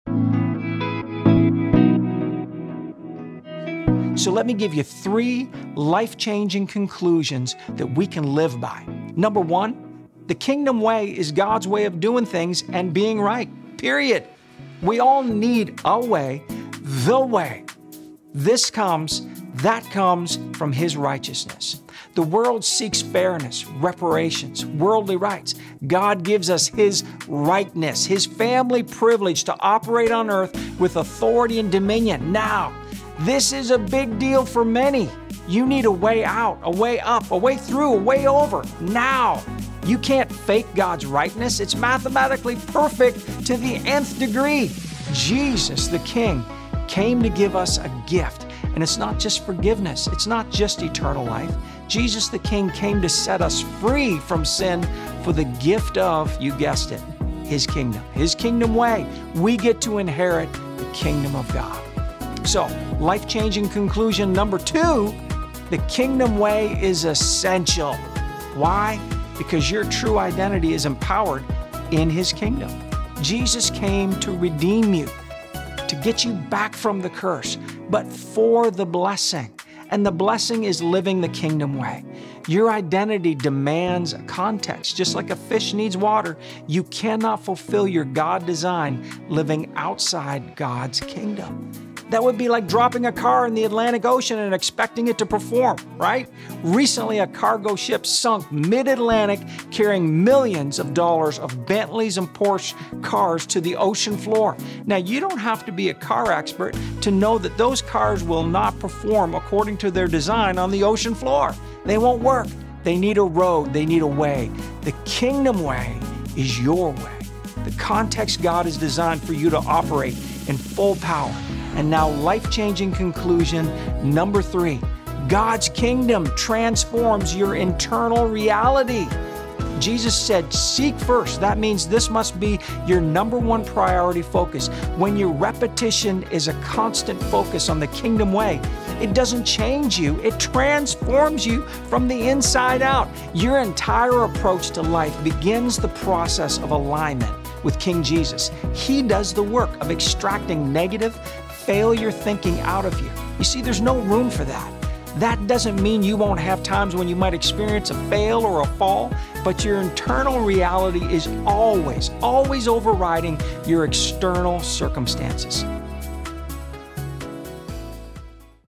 3 LIFE CHANGING CONCLUSIONS // Pastor Stephen Marshall
3 LIFE-CHANGING CONCLUSIONS
Your hurts and dreams are important to us that is why we want to encourage you.
Let me give you 3 life-changing conclusions that we can live by.
#1 THE KINGDOM WAY IS GOD'S WAY OF DOING THINGS AND BEING RIGHT.
We all need a way.. we need The Way! That comes from His Rightness.
The world seeks fairness, reparations, and worldly rights but wants to give us His Rightness and His family privilege to operate on earth with authority and dominion. Now, this is a big deal. You need a Way out a Way up a Way through- a Way over right now.
You can't fake God's Rightness its mathematically perfect to the  nth degree!
Jesus the King of Kings came to give us a gift. It's not just forgiveness. It's not just eternal life. Jesus came to set us free from the law of sin to the gift of His Kingdom. His Kingdom Way. We get to inherit the Kingdom of God.
#2 THE KINGDOM WAY IS ESSENTIAL.
Your true identity is empowered in His Kingdom. Jesus came to redeem you get you back from the curse FOR the Blessing! The Blessing is walking in the Way of God so you can receive the good Will of God. Your identity demands a context. Just like a fish needs water you cannot fulfill God's Will living outside God's Kingdom - His Way of thinking and doing things. You cannot drop a regular car in the middle of the Atlantic ocean and expect it to perform like it was designed to.Even a  Bentley needs a road and a way to perform correctly and in the ocean it will fail. The Kingdom Way can be your way, It is the context God designed you to operate in full power!
GOD'S KINGDOM TRANSFORMS YOU ETERNAL REALITY.
Jesus said " Seek first the KINGDOM Way and all good things would be added unto you"
This must be your number priority focus. When your Repetition is a constant focus on the Kingdom Way is doesn't change you it transforms you from the inside out! Your entire process in life begins the alignment with King Jesus. He does the work of extracting failure negative thinking  out of you. You might still experience a hurt- a fail or a fall but internal reality is always over riding  your external circumstances.
PRAY:
God, I humble myself now. I let go of my way of thinking and doing things. My hurts- my walls of protection- my fear and I say God i want Your Way!. Transform me and empower my identity in you Jesus so I can receive your Good and amazing Will for my life!